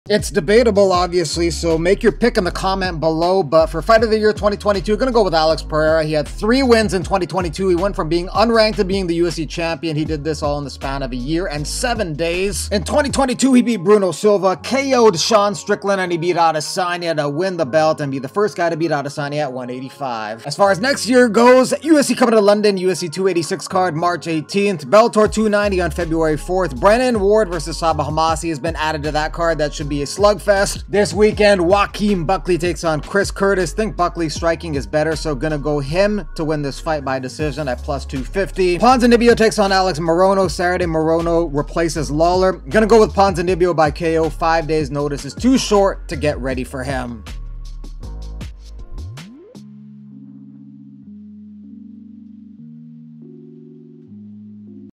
Fighter Of The Year...
It's debatable obviously , so post your pick in the comments below. But for Fighter of the Year 2022 will go with Alex Pereira.
3 wins in 2022, he went from being uranked to becing a UFC champion. He did this in the span of year and 7 days.
In 2022 he beat Bruno Silva, KO'd Strickland, and he beat Adesanya to win the belt; the first guy to beat Adesanya at 185

As far as next year goes...
UFC coming to London for the UFC 286 card on March 18th
Sabah Homasi vs Brennan Ward Feburary 4th at Bellator 290. It'll be a slugfest of a fight.

This weekend.
Joaquin Buckley vs Chris Curtis. I think Buckey's a better striker and this is going to be all standup, so picking Buckley by decision +250
Ponzinibbio is taking on Alex Morono on Saturday, Morono replaces Lawler.  . I'll go with Ponzinibbio by KO, 5 days notice is too short to get
ready for him.